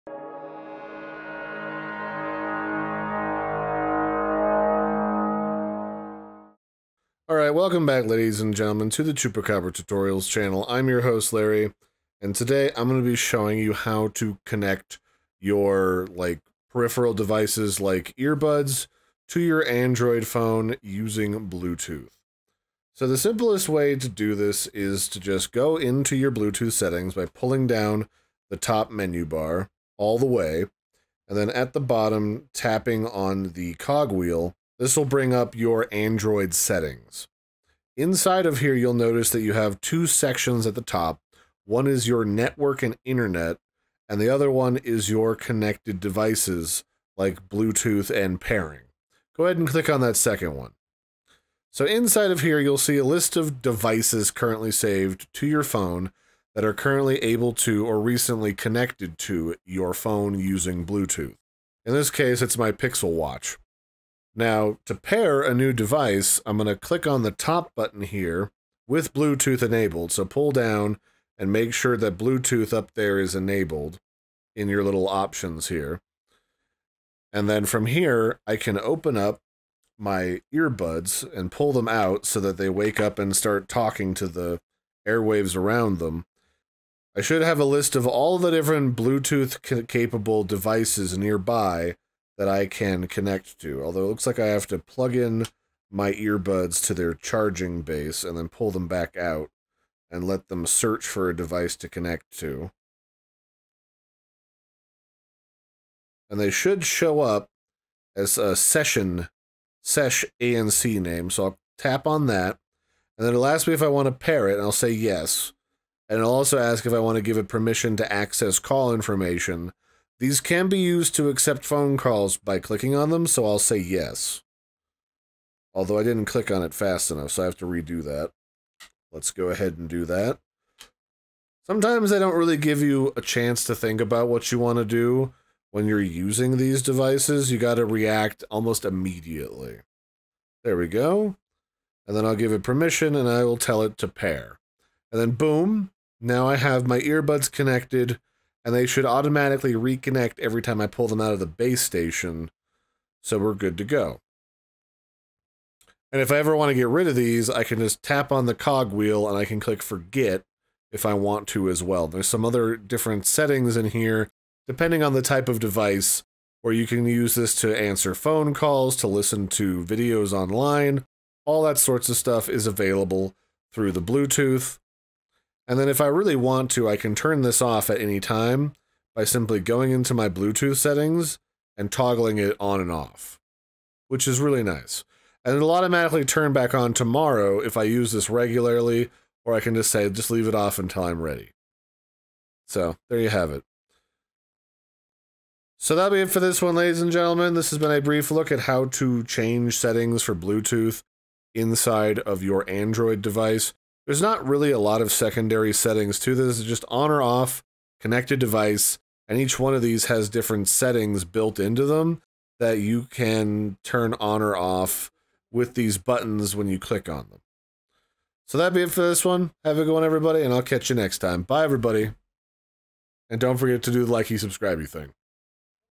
How to Use Android Bluetooth Settings & Pair Earbuds (2025 Updated) 🎧📶
Need to pair Bluetooth earbuds or manage Bluetooth settings on your Android phone? 📱🔊 This guide will show you step-by-step how to connect Bluetooth devices, adjust settings, and fix common pairing issues using Android’s latest Bluetooth features in 2025!

Whether you're setting up wireless headphones, speakers, or car audio, this tutorial makes it fast & easy to connect your devices! 🚀

🔹 In This Video:
✅ How to Turn On & Access Bluetooth Settings 🔄
✅ How to Pair Bluetooth Earbuds & Headphones 🎧
✅ Managing Connected Devices & Audio Preferences 🎛️
✅ Fixing Common Bluetooth Connection Issues 🔧